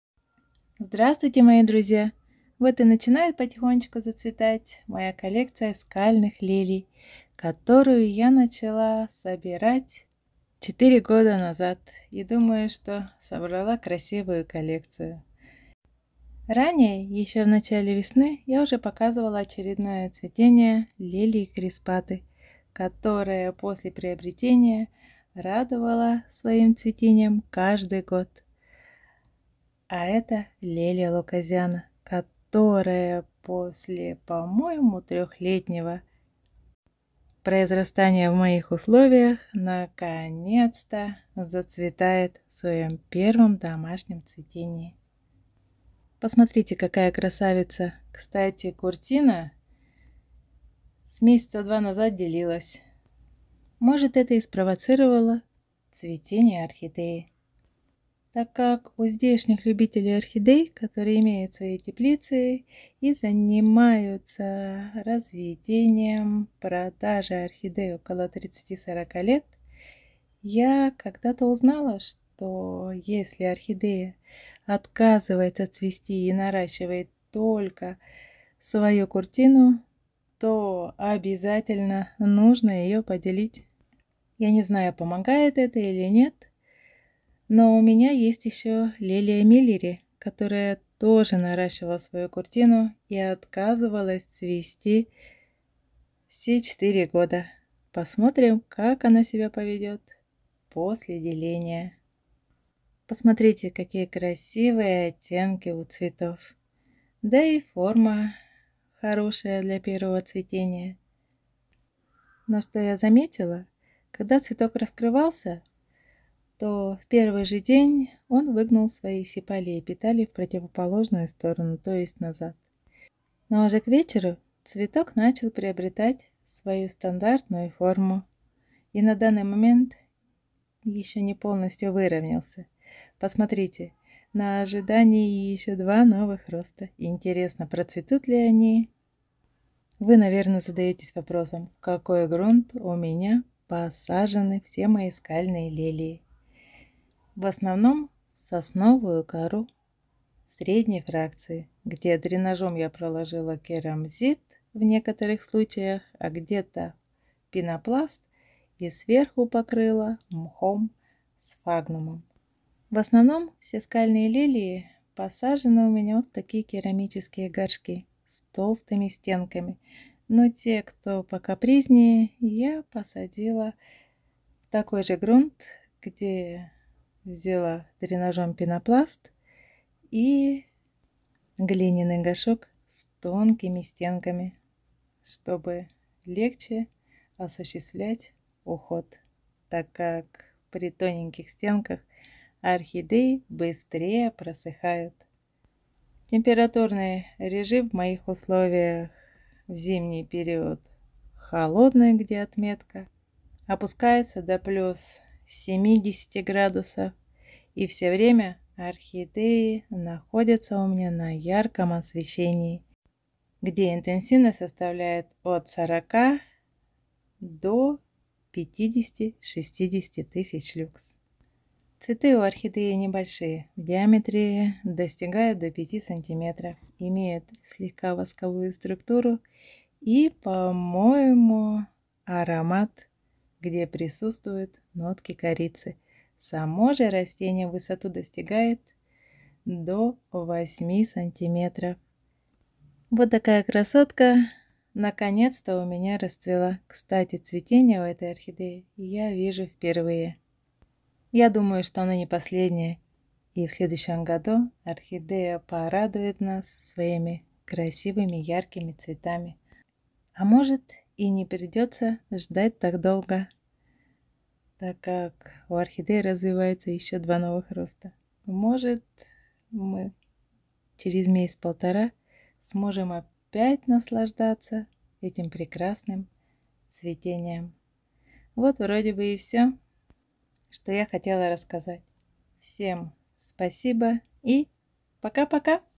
Laelia lucasiana наконец-то зацвела...
Найден в горах штата Минас-Жерайс в Бразилии на высоте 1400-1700 метров в виде миниатюрного прохладнорастущего литофита на полностью обнаженных каменистых выходах с приземистыми грушевидными псевдобульбами, несущими одиночный эллиптический лист на вершине, который распускается на  прямостоячая, 2 1/4 дюйма [6 см] в длину, кисть с небольшим [от 1 до 3] цветком, которая только сбрасывает высоту листа, встречается поздней весной и летом при выращивании и осенью в Бразилии.